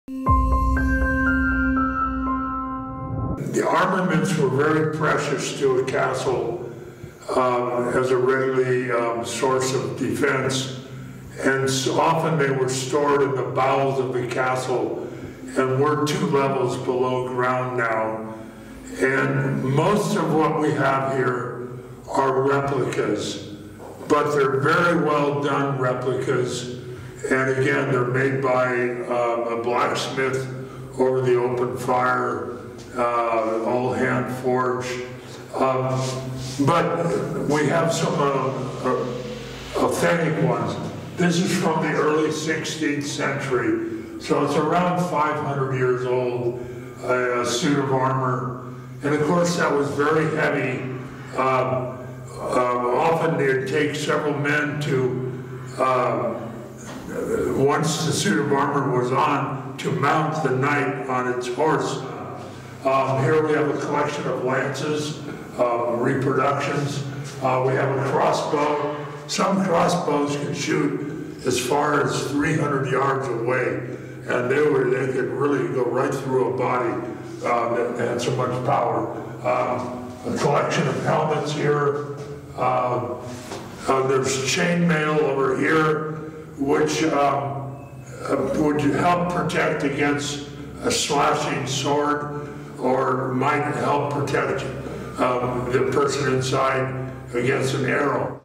Examine The Armament Room & Medieval Weapons of Castello di Amorosa.
Examine the medieval weapons & armor in Castello di Amorosa's armament room with owner Dario Sattui. You’ll find suits of armor and weapons dating back hundreds of years. You can’t miss the Pit of Despair, a dungeon cell with an opening at the top!